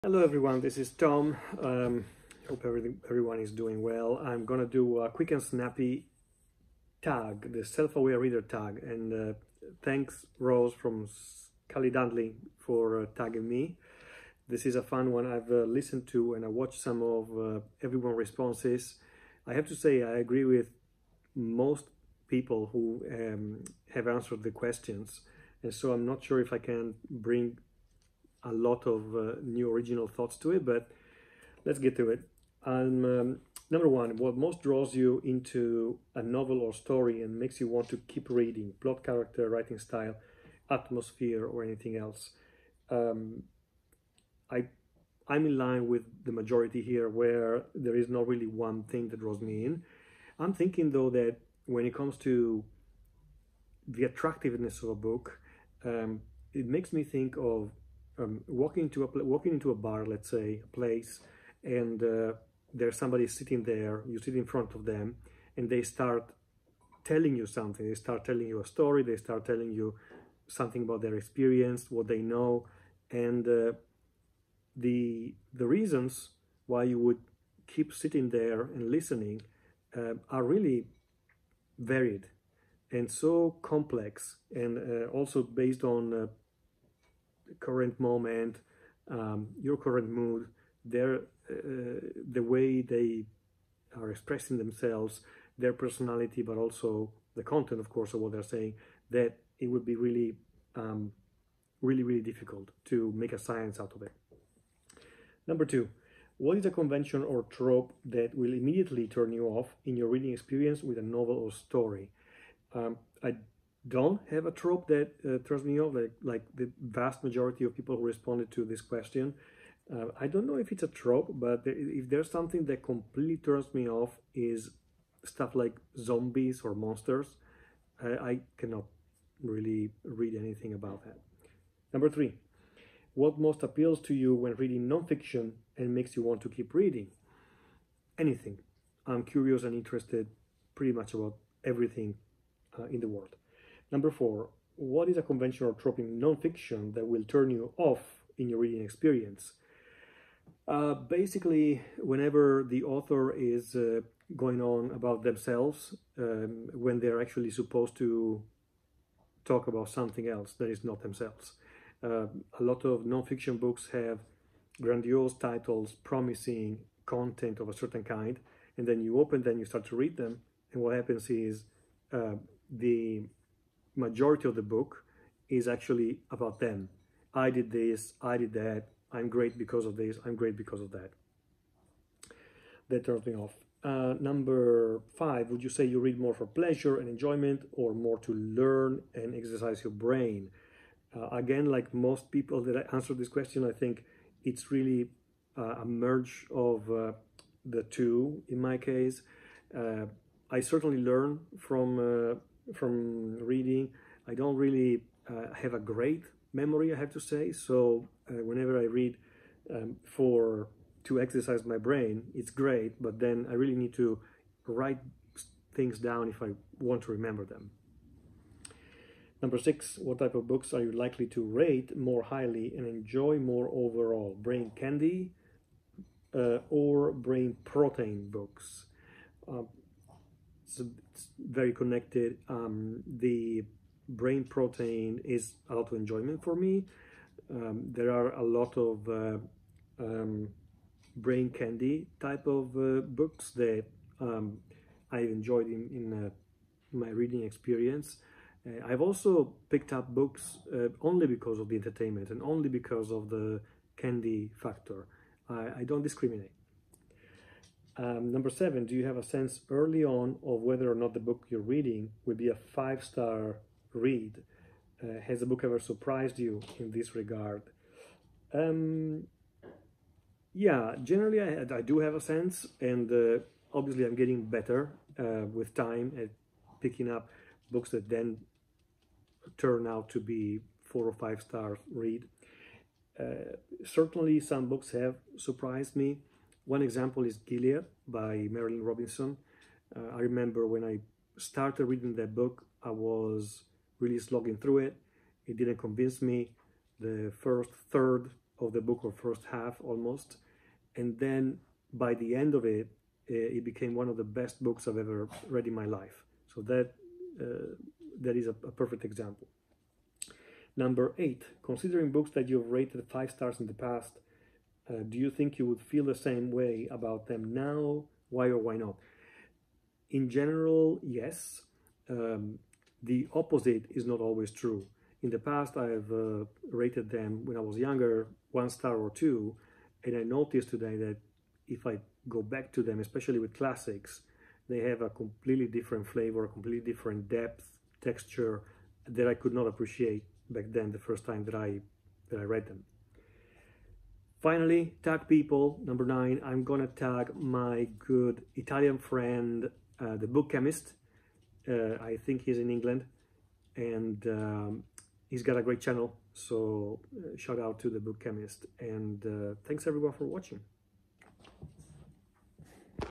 The Self-Aware Reader Tag
My neighbor is busy drilling a hole to the center of the Earth, so I thought it would be wise to record this video inside.

Prompts:
1) What most draws you in to a novel or story and makes you want to keep reading - plot, character(s), writing style, atmosphere, something else?
2) What is a convention or trope that will immediately turn you off in your reading experience with a novel or story?
3) What most appeals to you when reading nonfiction and makes you want to keep reading?
4) What is a convention or trope in nonfiction that will turn you off in your reading experience?
5) Would you say you read more for pleasure and enjoyment or more to learn and exercise your brain?
6) Which type of books are you likely to rate more highly and enjoy more overall - brain "candy" (pleasure/enjoyment) books or brain "protein" (learning/exercise) books?
7) Do you have a sense early on of whether or not the book you're reading will be a five-star read (or a book you will really like)? Has a book ever surprised you in this regard?
8) Considering books that you've rated five stars in the past (or if you don't rate them, then books you really loved), do you think you would feel the same way about them now? Why or why not?
9) Tag people.

Tom